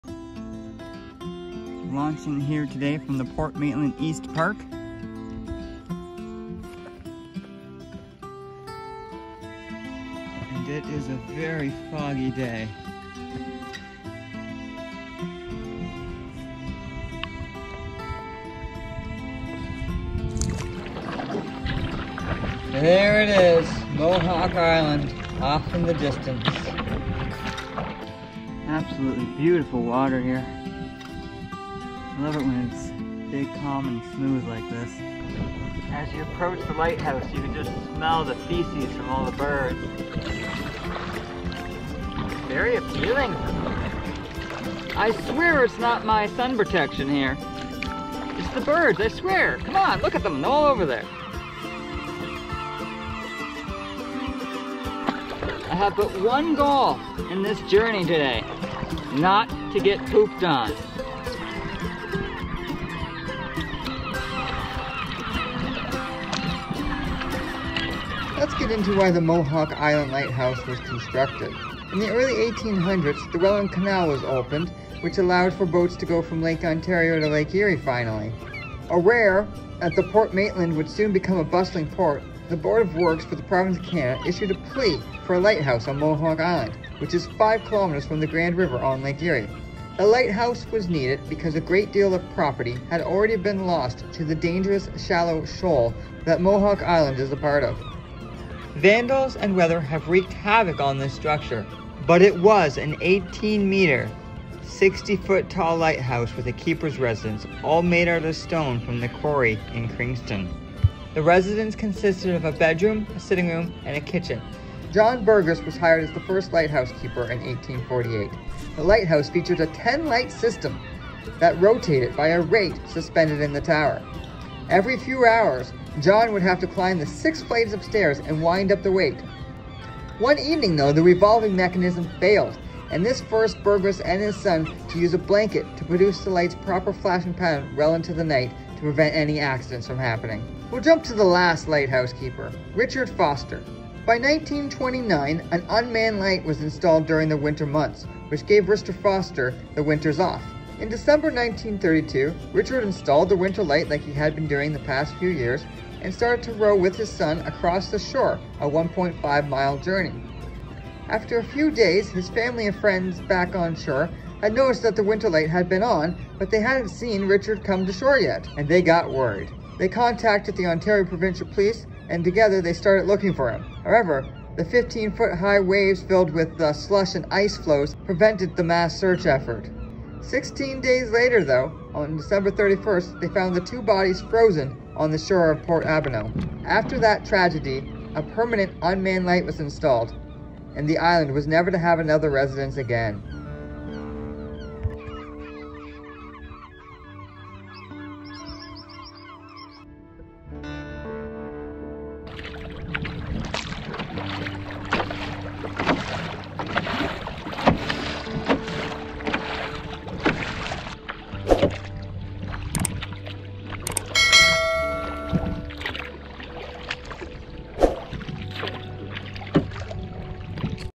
Paddling to Mohawk Island Lighthouse
I paddled from Port Maitland East Park to Mohawk Island Lighthouse in Lake Erie.